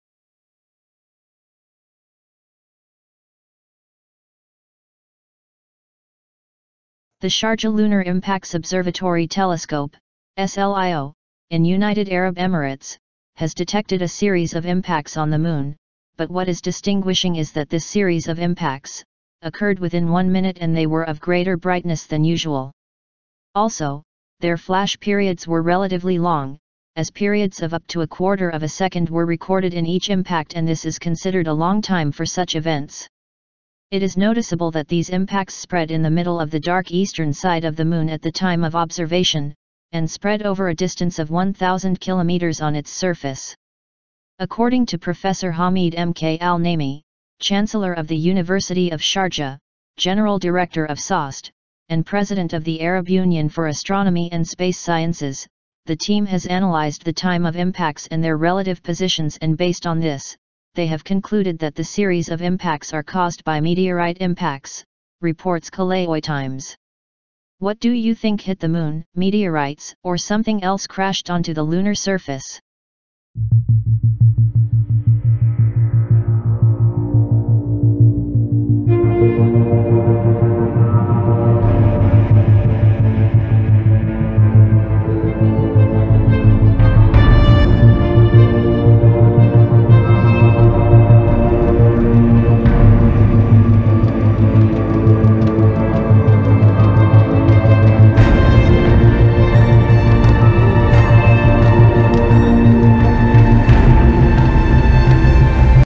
Sharjah Observatory captured a series of rare impacts on the lunar surface
The Sharjah Lunar Impacts Observatory Telescope (SLIO). in United Arab Emirates has detected a series of impacts on the moon, but what is distinguishing is that this series of impacts, occurred within one minute and they were of greater brightness than usual.